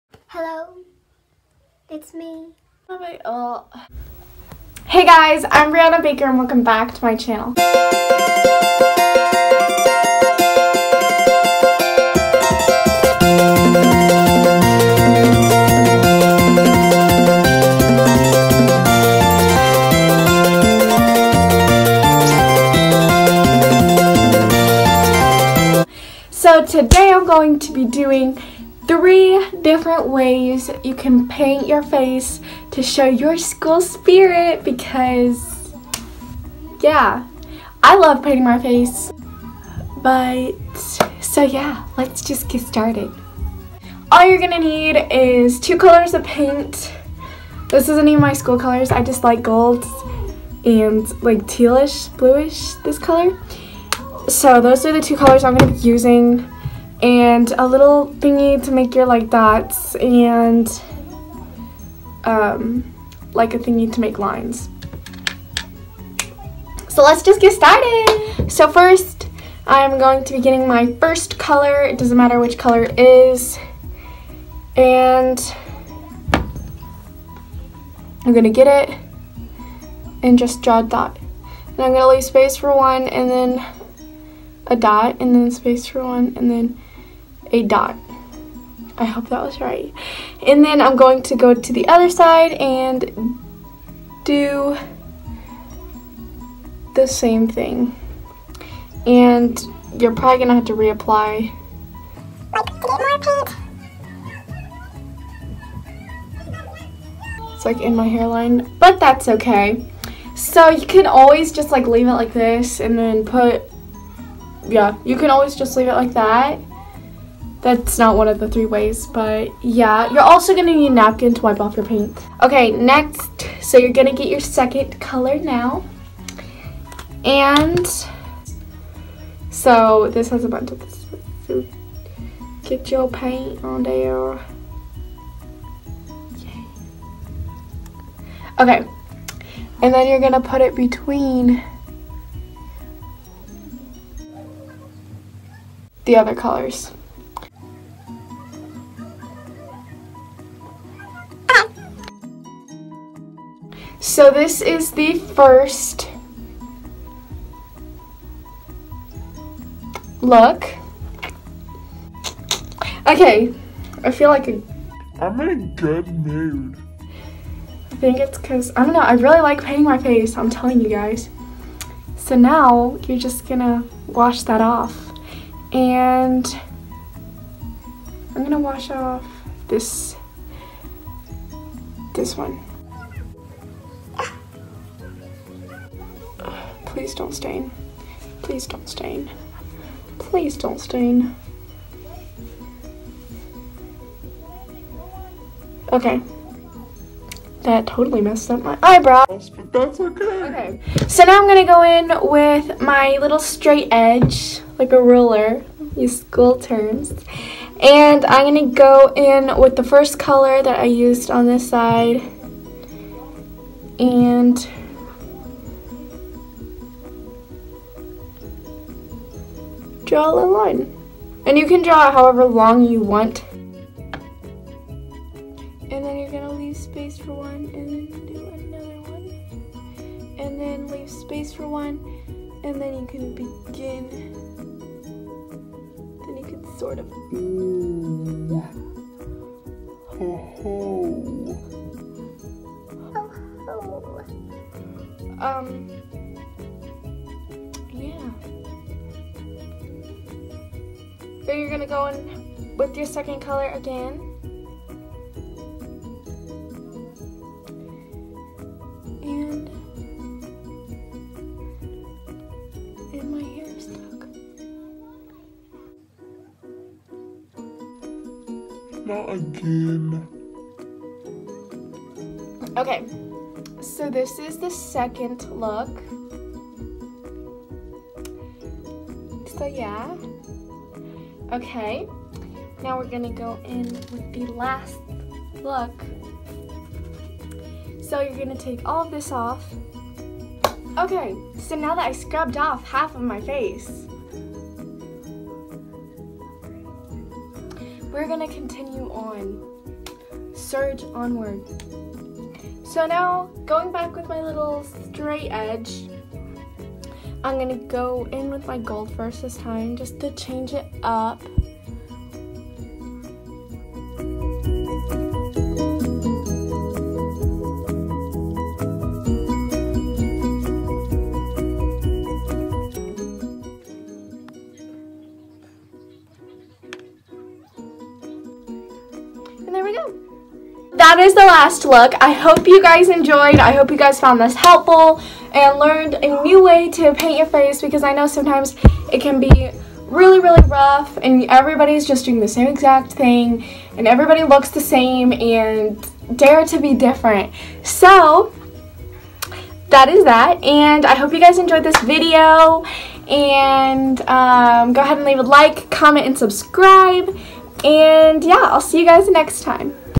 SCHOOL SPIRIT FACE PAINT (different)
I’m always trying to find new ways to paint My face, so I thought I’d give you guys some ideas. Hope you enjoy!
Xoxo brianna